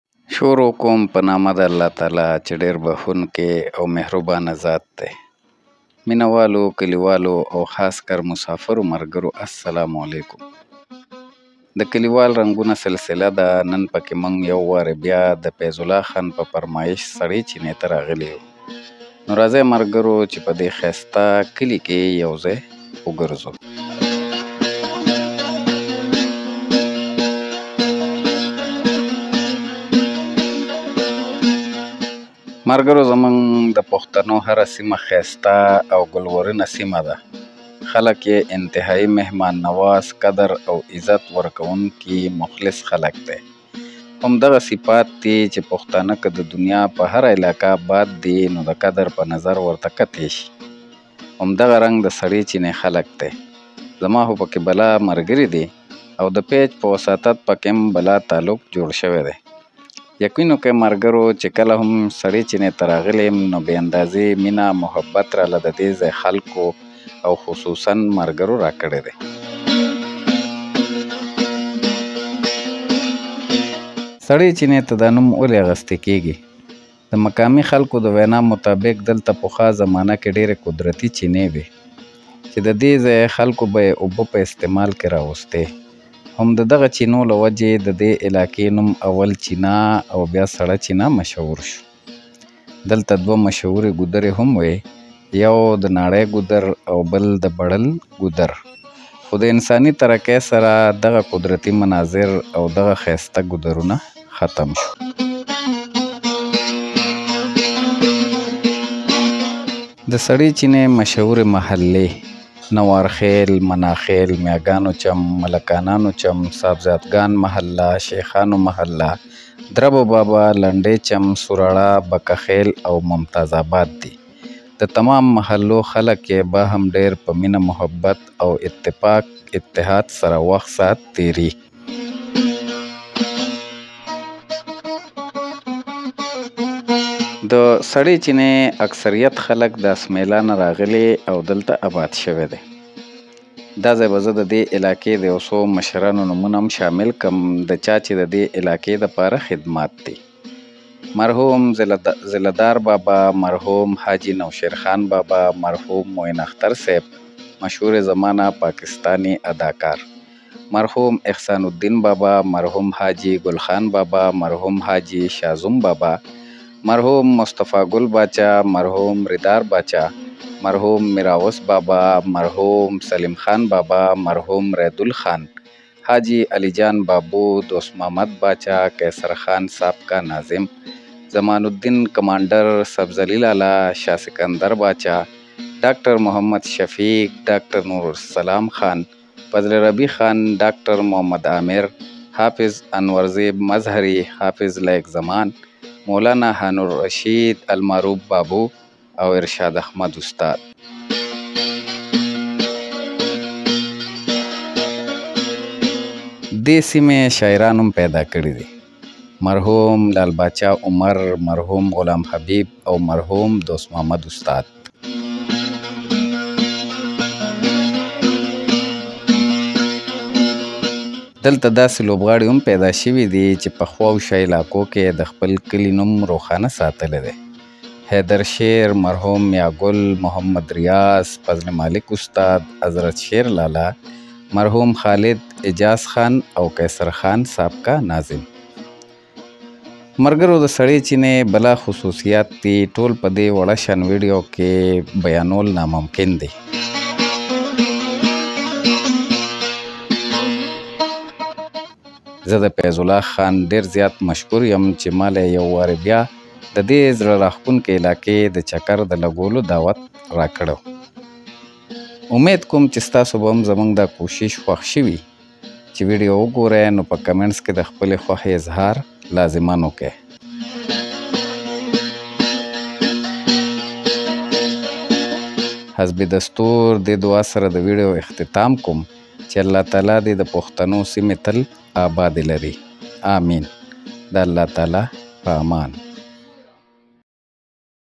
The Land of Beautiful people | Sard cheena (راځے ملگرو چہ د سڑے چینے چکر اولگو)
ملگرو سڑہ ڈیر ښائستہ او زڑہ راخکونکے کلے دے ، راځے مونږ سرہ چہ پہ ښکلے کلی کښې یوزئے چکر اولگو۔